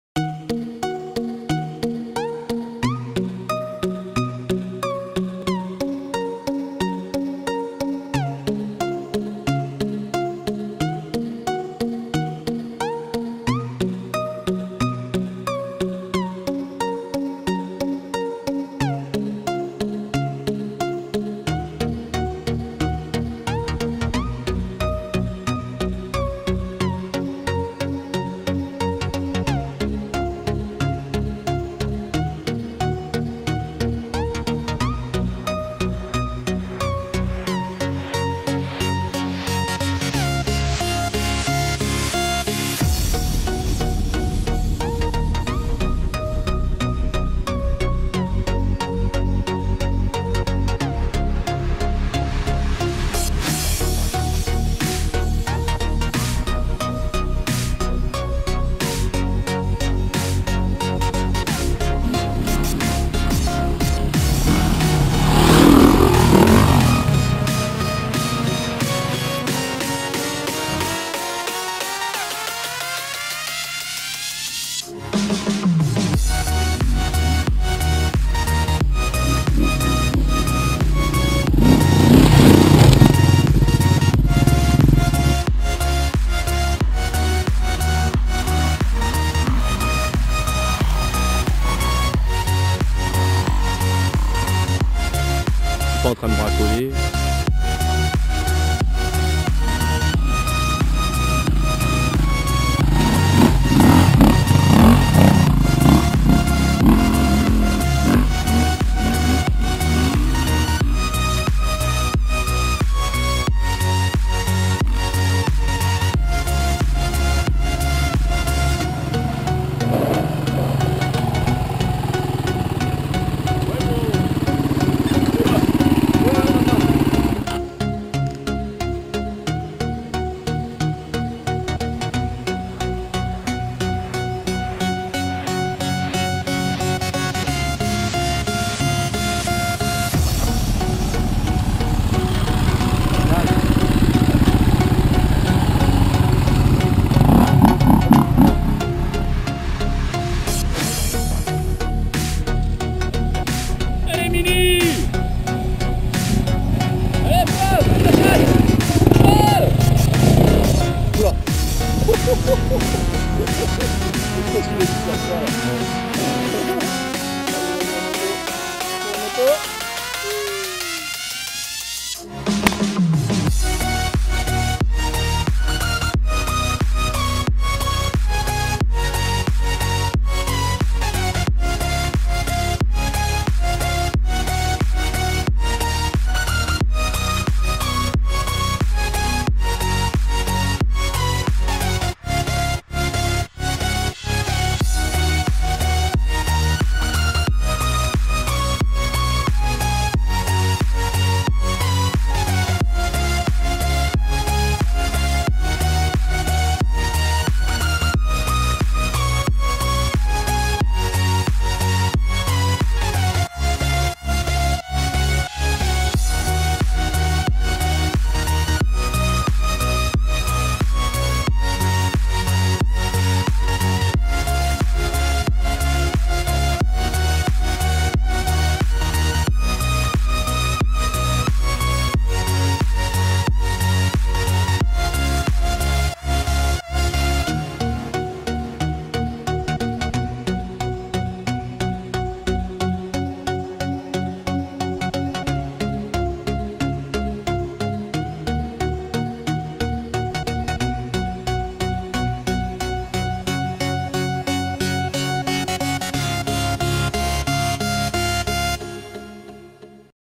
ISSOIRE MOTO QUAD ENDURO
sortie du 05/08/2017 Le Vernet-La-Varenne 80 km de pur enduro